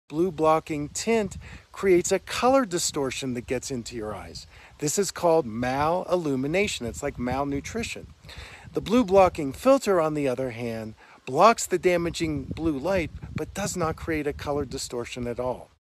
Light is a food!
Receiving the proper light into the eyes is as important as eating healthy, organic, non-GMO food.  A balanced amount of light improves mitochondria function which gives us more energy, and removes toxins from our cells.